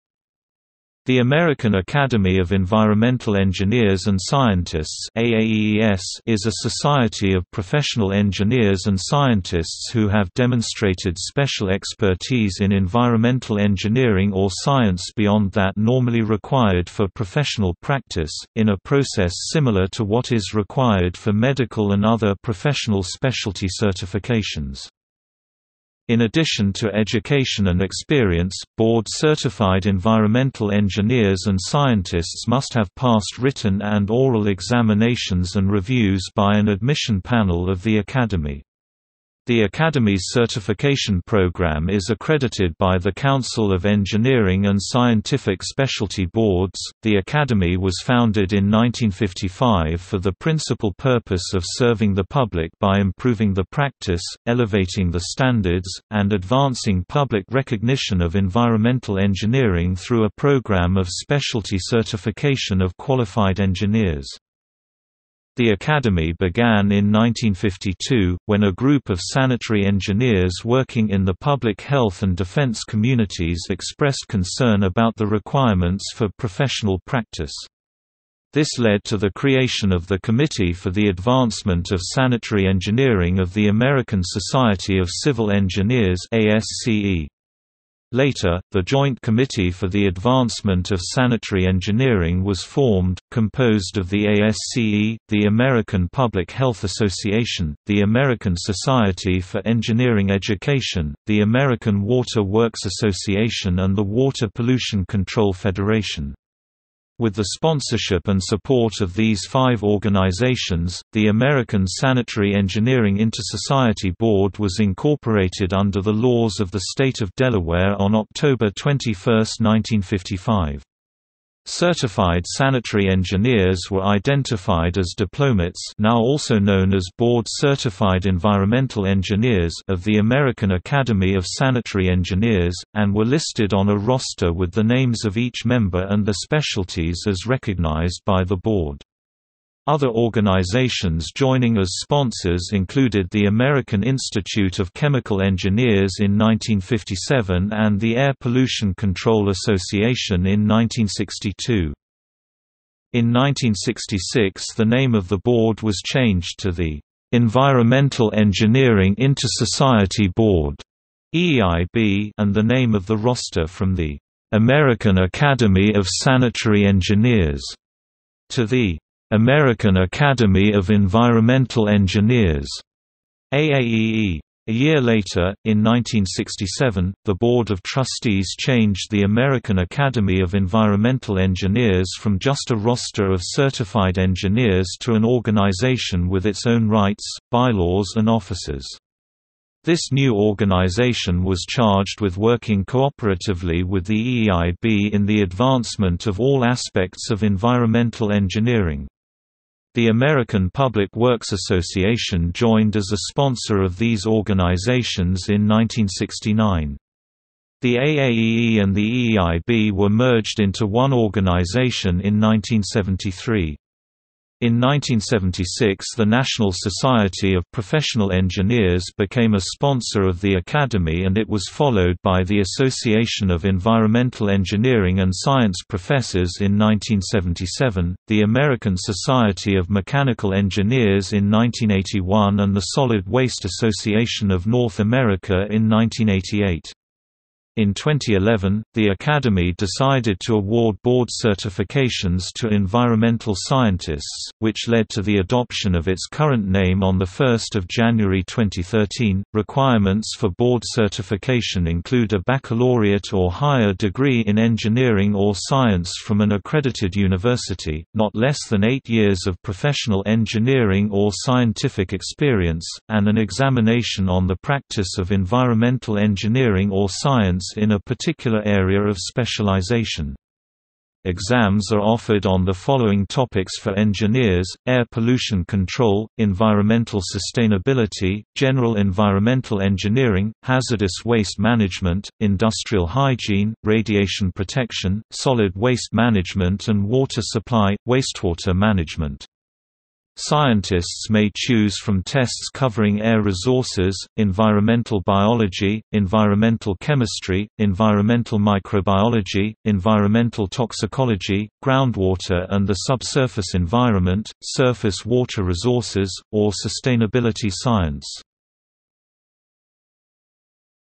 American Academy of Environmental Engineers and Scientists | Wikipedia audio article
- Socrates

SUMMARY
The American Academy of Environmental Engineers and Scientists (AAEES) is a society of professional engineers and scientists who have demonstrated special expertise in environmental engineering or science beyond that normally required for professional practice, in a process similar to what is required for medical and other professional specialty certifications.
In addition to education and experience, Board-Certified Environmental Engineers and Scientists must have passed written and oral examinations and reviews by an admission panel of the Academy. The Academy's certification program is accredited by the Council of Engineering and Scientific Specialty Boards.The Academy was founded in 1955 for the principal purpose of serving the public by improving the practice, elevating the standards, and advancing public recognition of environmental engineering through a program of specialty certification of qualified engineers.
The Academy began in 1952, when a group of sanitary engineers working in the public health and defense communities expressed concern about the requirements for professional practice. This led to the creation of the Committee for the Advancement of Sanitary Engineering of the American Society of Civil Engineers (ASCE). Later, the Joint Committee for the Advancement of Sanitary Engineering was formed, composed of the ASCE, the American Public Health Association, the American Society for Engineering Education, the American Water Works Association and the Water Pollution Control Federation. With the sponsorship and support of these five organizations, the American Sanitary Engineering Intersociety Board was incorporated under the laws of the State of Delaware on October 21, 1955. Certified sanitary engineers were identified as Diplomates (now also known as Board Certified Environmental Engineers) of the American Academy of Sanitary Engineers, and were listed on a roster with the names of each member and their specialties as recognized by the Board. Other organizations joining as sponsors included the American Institute of Chemical Engineers in 1957 and the Air Pollution Control Association in 1962.
In 1966 the name of the Board was changed to the "Environmental Engineering Intersociety Board" (EEIB) and the name of the roster from the "American Academy of Sanitary Engineers" to the "American Academy of Environmental Engineers" (AAEE). A year later, in 1967, the Board of Trustees changed the American Academy of Environmental Engineers from just a roster of certified engineers to an organization with its own rights, bylaws and officers. This new organization was charged with working cooperatively with the EEIB in the advancement of all aspects of environmental engineering. The American Public Works Association joined as a sponsor of these organizations in 1969. The AAEE and the EEIB were merged into one organization in 1973. In 1976 the National Society of Professional Engineers became a sponsor of the Academy and it was followed by the Association of Environmental Engineering and Science Professors in 1977, the American Society of Mechanical Engineers in 1981 and the Solid Waste Association of North America in 1988. In 2011, the Academy decided to award board certifications to environmental scientists, which led to the adoption of its current name on 1 January 2013.Requirements for board certification include a baccalaureate or higher degree in engineering or science from an accredited university, not less than eight years of professional engineering or scientific experience, and an examination on the practice of environmental engineering or science in a particular area of specialization. Exams are offered on the followi ...